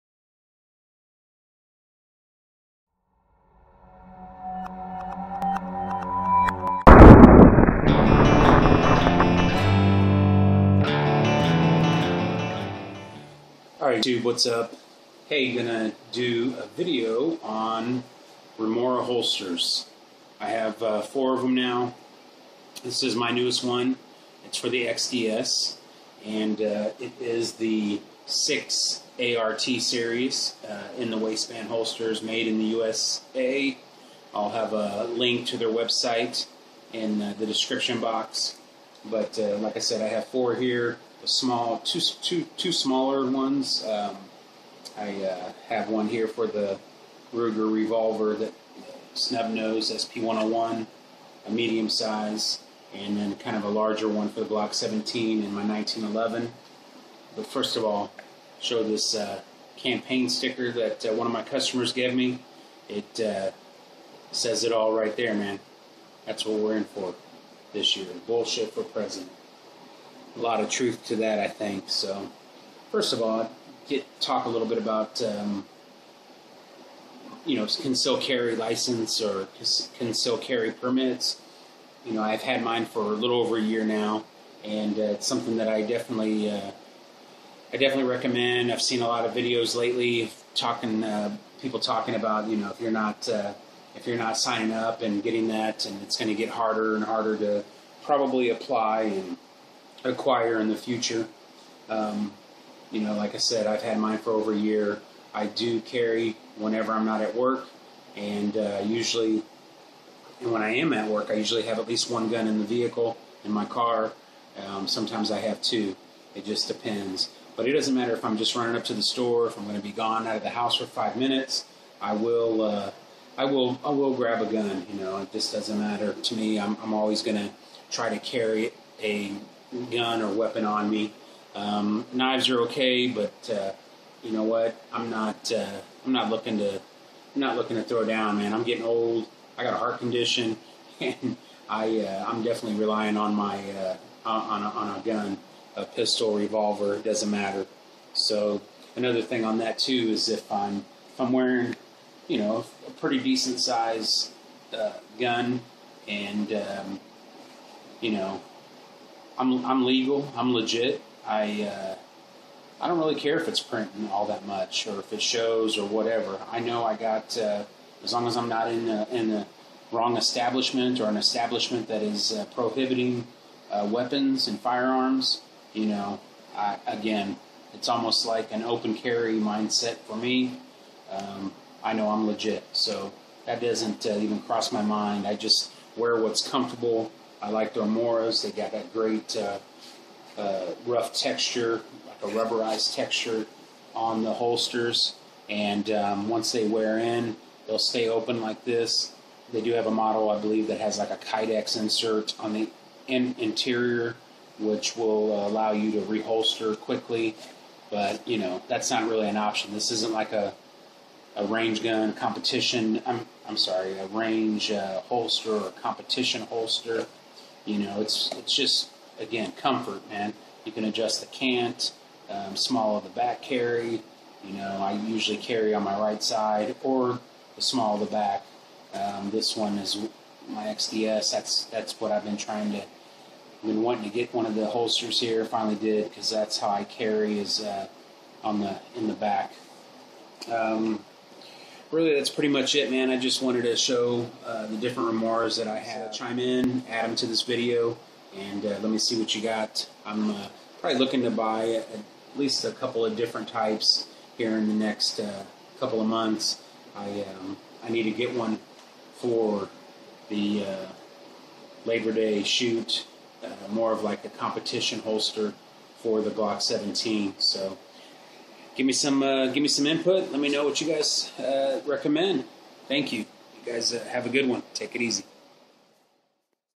Best Gun Holster: Remora Holsters Fat Guy Carry
I show my Remora holsters that I use for CCW. Thanks to ShotGunShanni and prchico520 for turning me on to the Remoras. Great set-ups and made in the USA at a great price$$$.